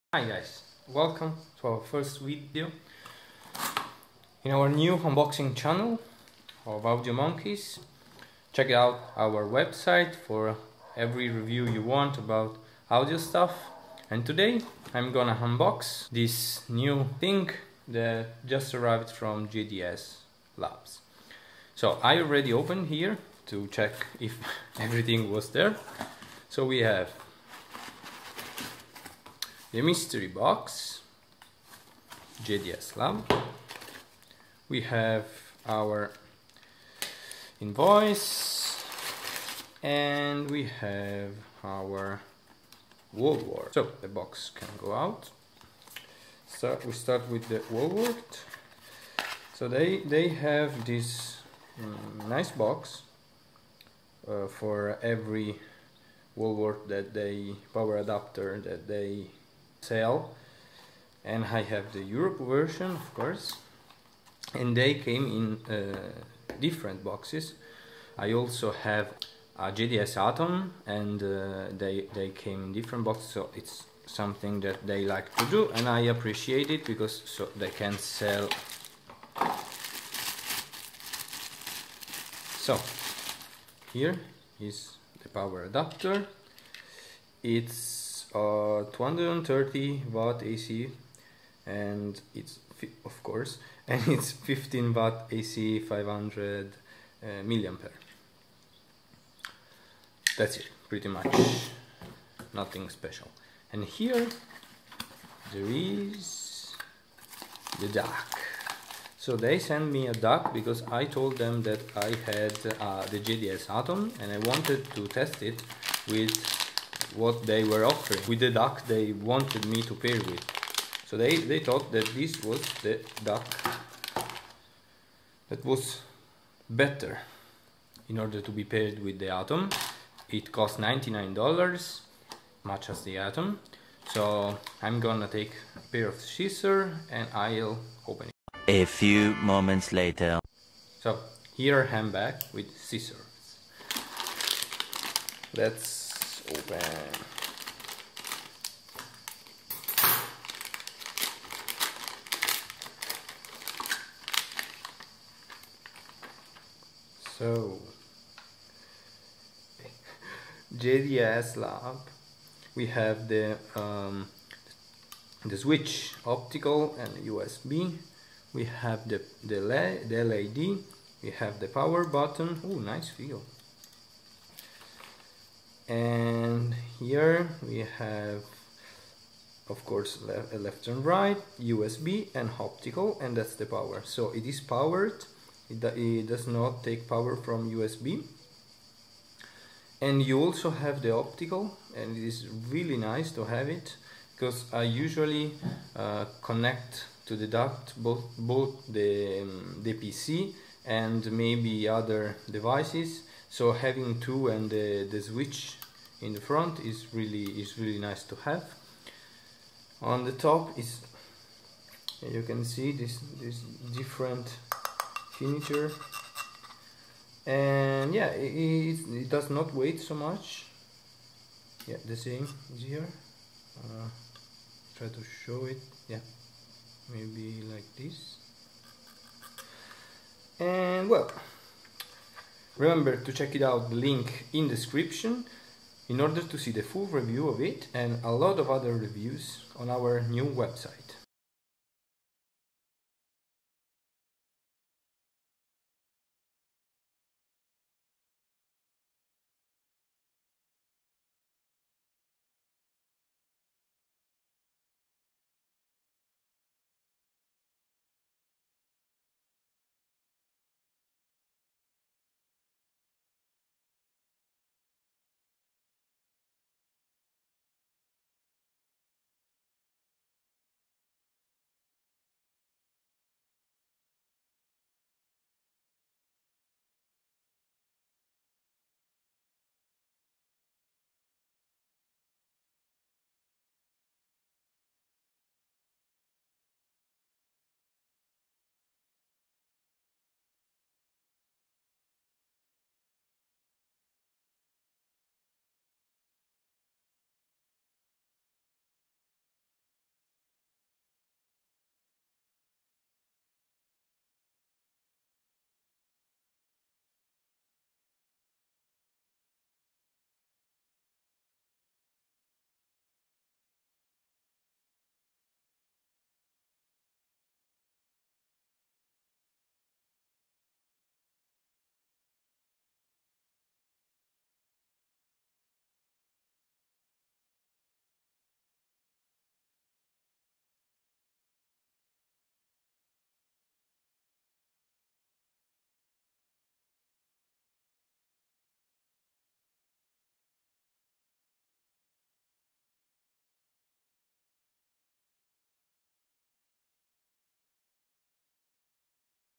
JDS OL DAC Unboxing
Today I am going to unbox the JDS OL DAC, a perfect DAC to match the JDS Atom or whatever you like. Stunning performance out of this little box, but no more spoilers for the full review